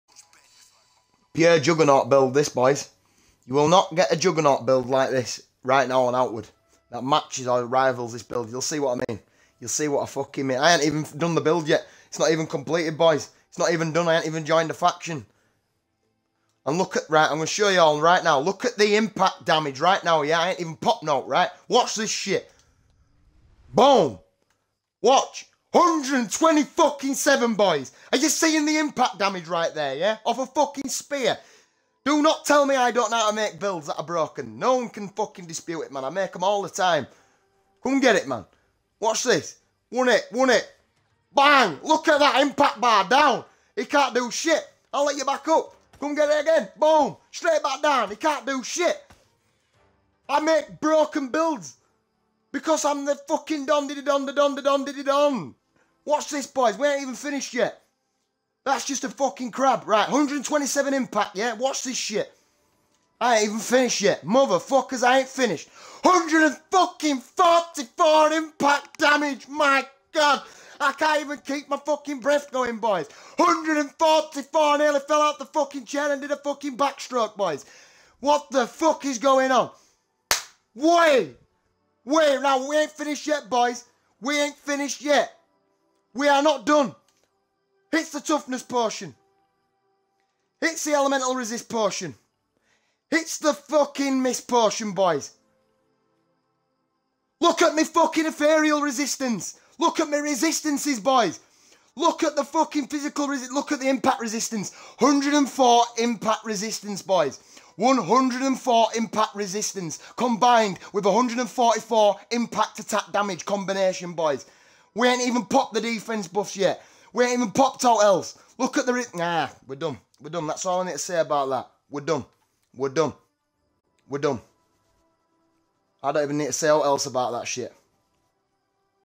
Vex Having a nerdfit over his new Blood Queen build on hc perma.
144 impact dmg. 104 impact resistance. the spear inflicts weaken. taking down the enemys Physical damage and impact damage it deals to you by -40%!